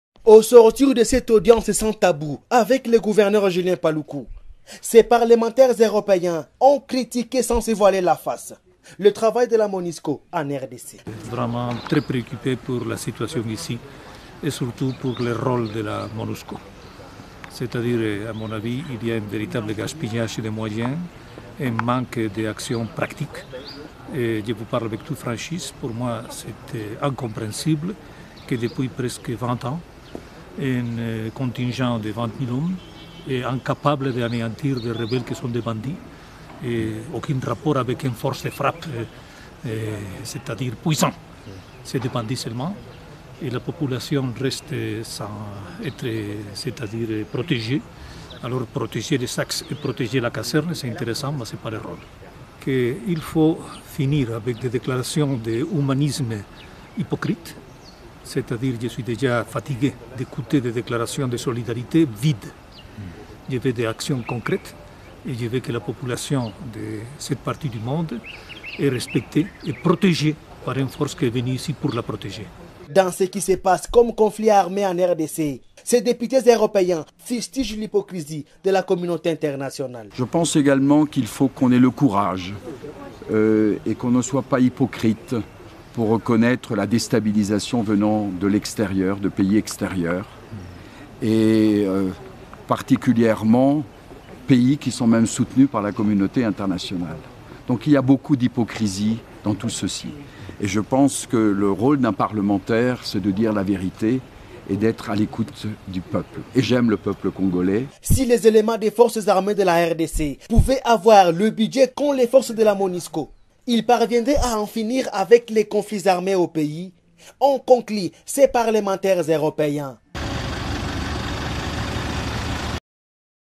Parlementaires européens dénoncent l'hypocrisie au Congo
Vidéo de Lwakale Mubengay BAFWA